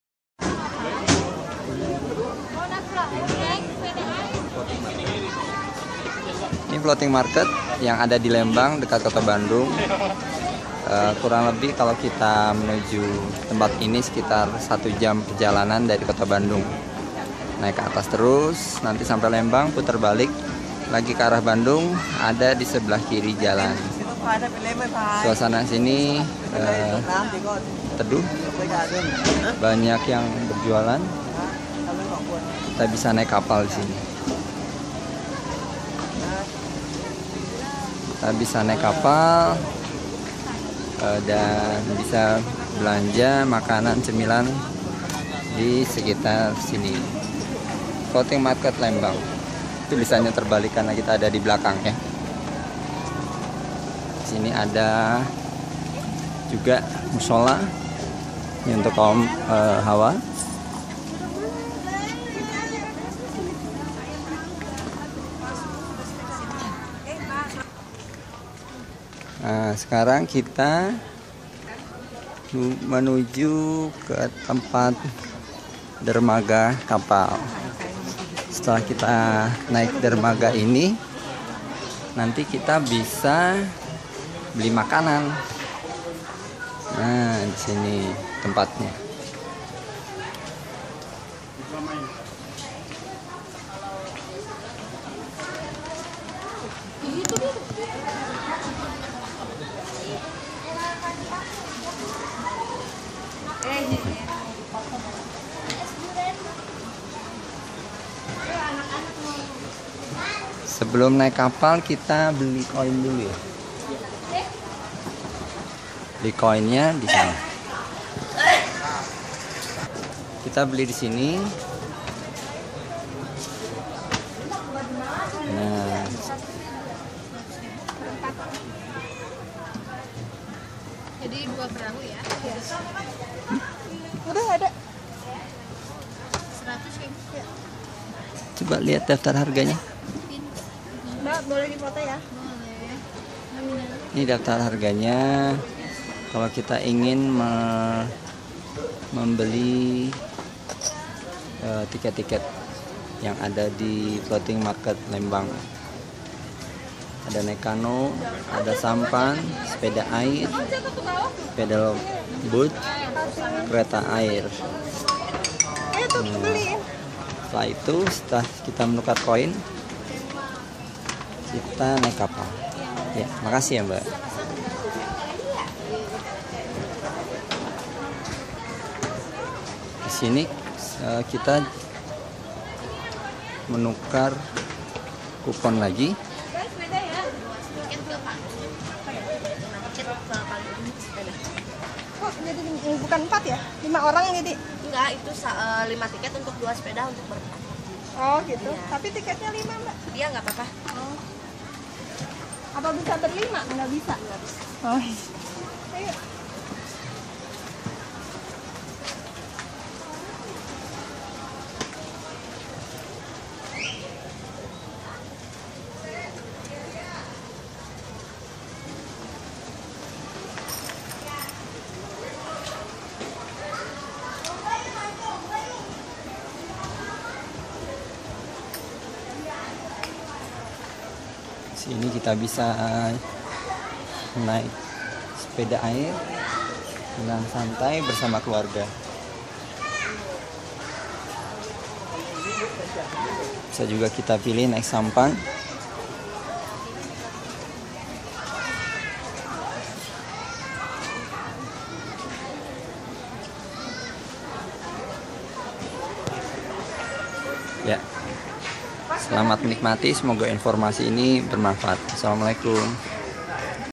Wisata Lembang Pasar Apung 🔵 Wisata Bandung Floating Market Lembang
Wisata Lembang Bandung Floating Market Lembang adalah tempat yang menarik dikunjungi bersama keluarga tercinta. Di sini kita bisa menyaksikan banyak pemandangan yang segar sambil menikmati secangkir kopi atau coklat yang disediakan untuk pengunjung dengan harga terjangkau.

Floating Market Wisata Lembang, menjadi salah satu tempat wisata rekomendasi dikunjungi bagi keluarga.